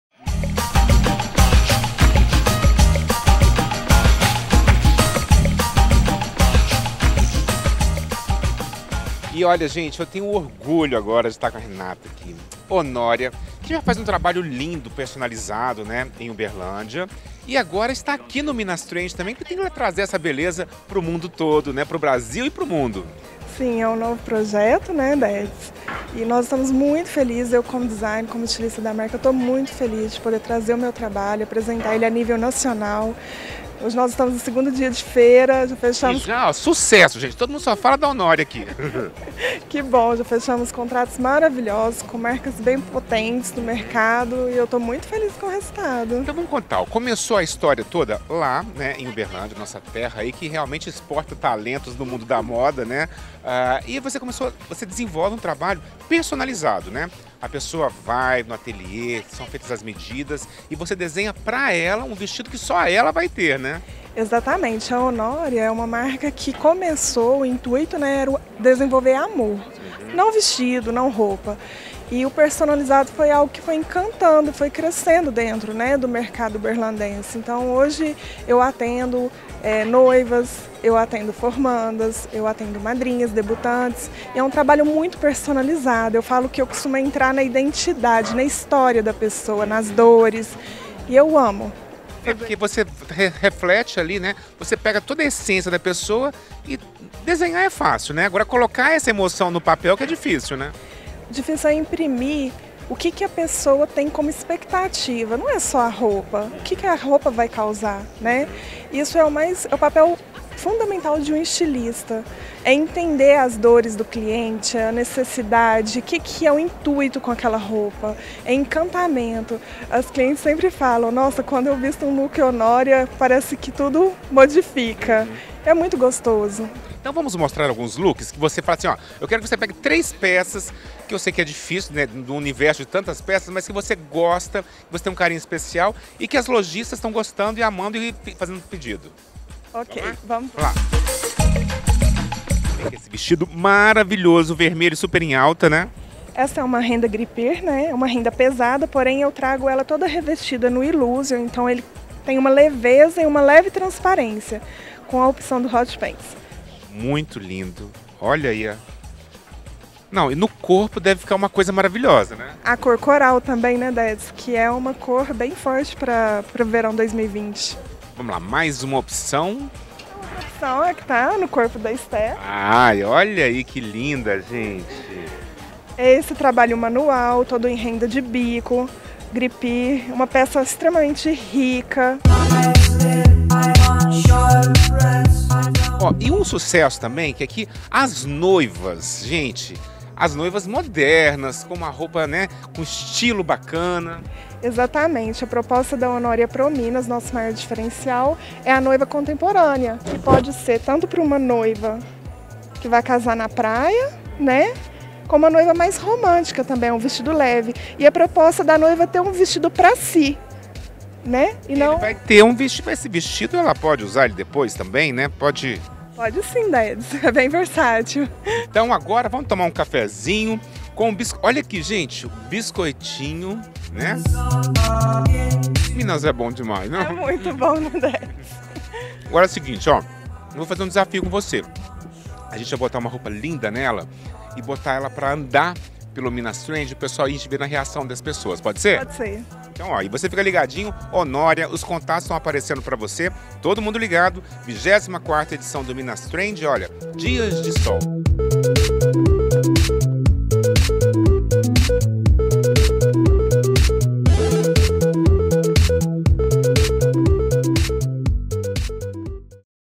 MINAS TREND EP 05: HONÓRIA 1930
O Minas Trend movimenta a moda festa de todo o país! A grife uberlandense Honória 1930, uma homenagem da estilista Renata Ribeiro à avó, estreou em BH apostando nos modelos de vestidos para noivas.